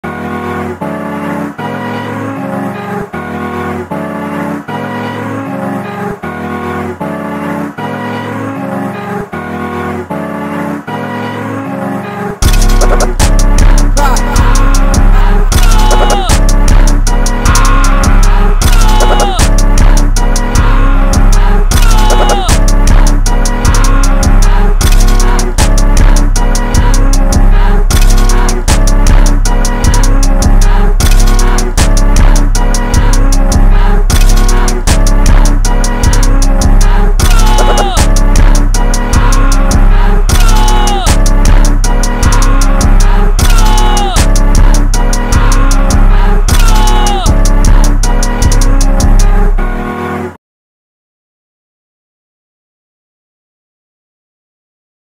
This might be my most sinful beat
Yes sorry if the beat was short I had to try and post something quick but I'll try to get own longer beats for yall. Also merry Christmas to everyone.

Also don't ask why I used prod.cry tag I just thought I went well with the beat 💀